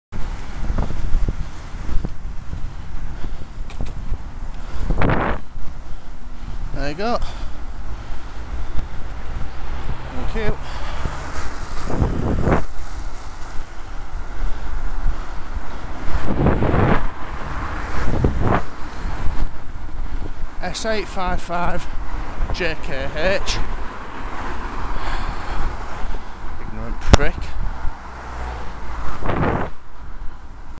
S855JKH - Impatient, close pass (and thank you, unknown Rover driver)
In hindsight I should have called out the Rover driver's reg, because it's not readable on the video, but thank you for waiting through the road narrowing and giving me a nice wide berth :)

On the other hand, S855JKH doesn't seem to understand that if someone's indicating right you're not supposed to pass them, nor that making someone pull their hand in so your wingmirror doesn't twat them that you're too close. I'll be keeping my eye out for this prick in the future.